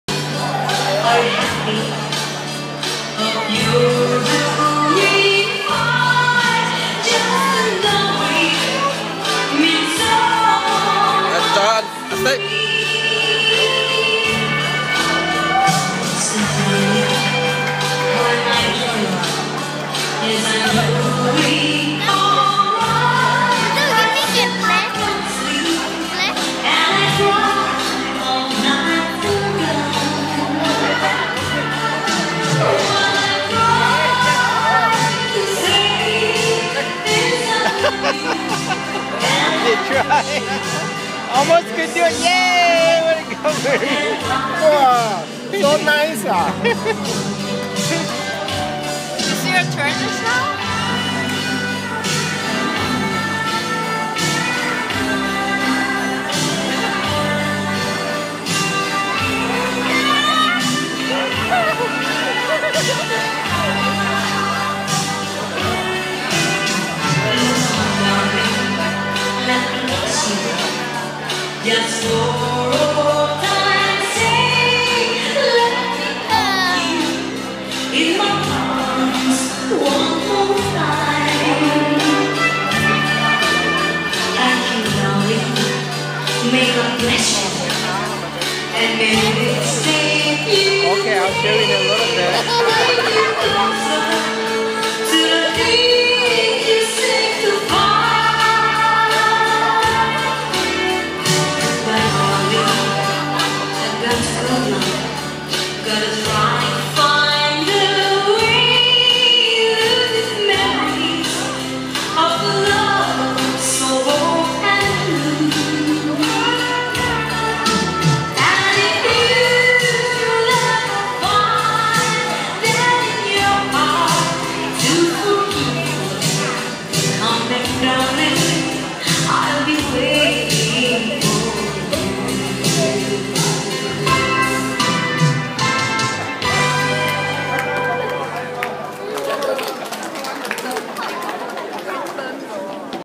Marie n Johnny take the dance floor
At 3rd aunt n uncle Ruby anniversary party in Miri, Malaysia. April 2012